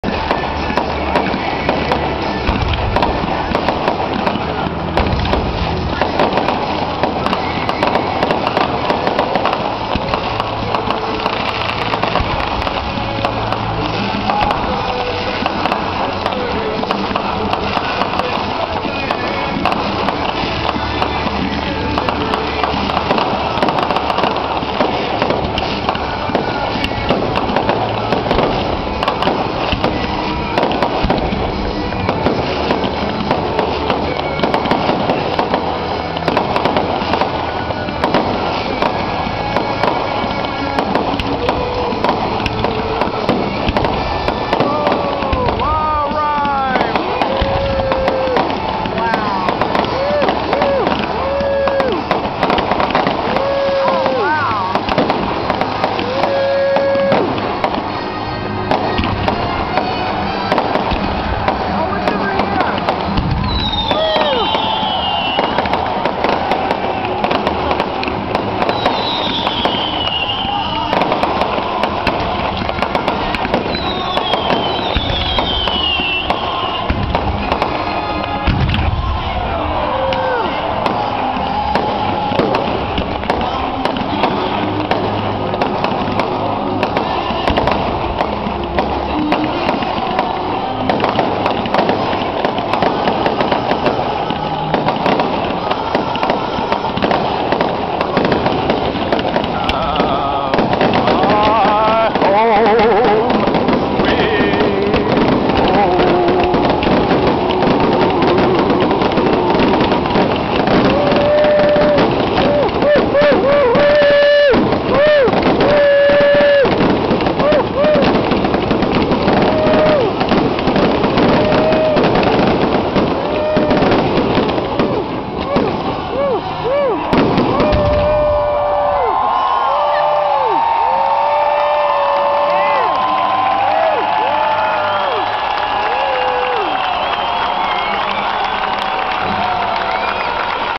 4th of July Fireworks Indianapolis Indians Baseball Game - 7/4/11 Part 3
Mary and I went to the Indianapolis Indians Baseball Game to see the Indians play. This was a special game! They had freebies and give aways! Plus a special fireworks show follwed by the downtown Indianapolis Fireworks!